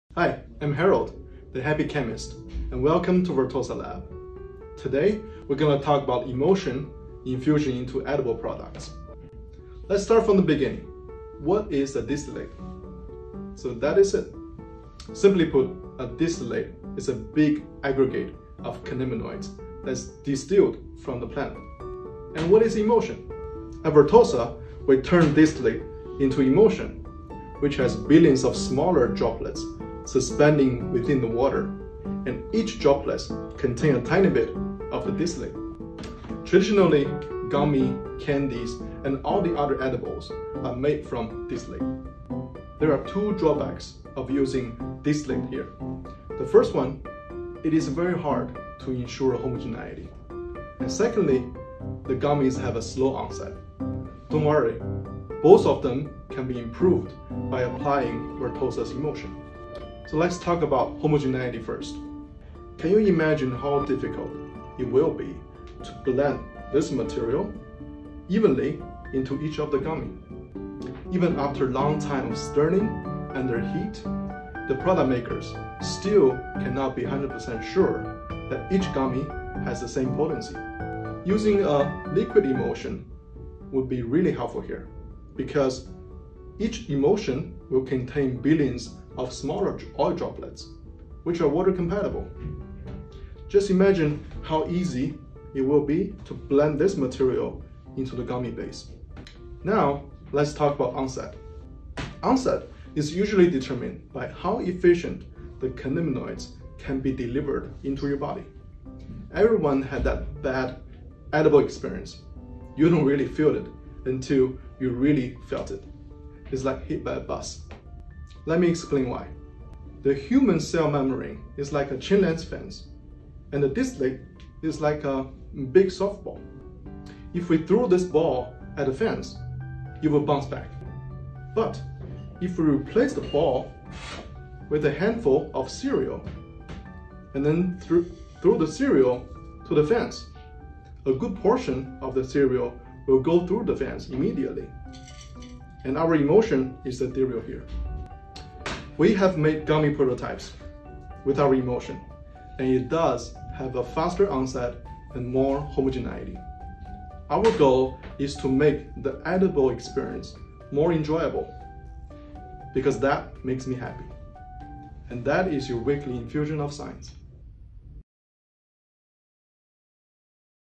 The Happy Chemist: Emulsion Infusion into Edible Products
Traditionally, gummies, candies and other edibles are often made with distillate but there are two drawbacks of using distillate, for one, it’s hard to ensure homogeneity, the equal dispersement of the distillate so that each piece is an accurate dose and the other is the gummies tend to have a slower onset time. 🌱

Learn more about emulsion systems for gummies, candies and edible products in this week's infusion of science! 🌱thehappychemist